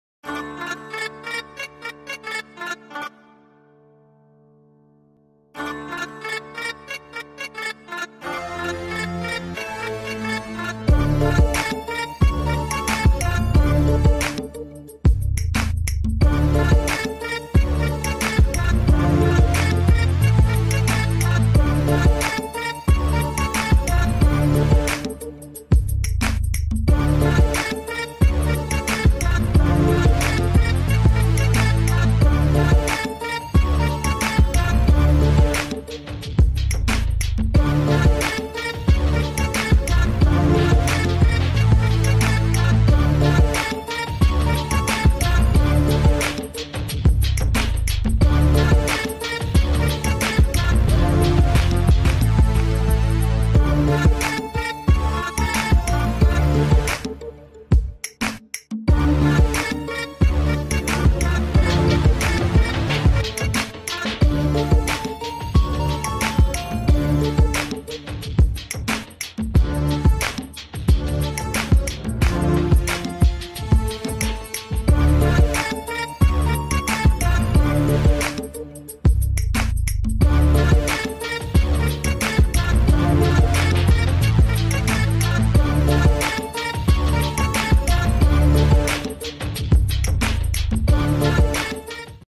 Lima Program Prioritas Nasional Menko Airlangga - Liputan6.com
Menko Airlangga menyebut 5 program prioritas nasional bidang perekonomian.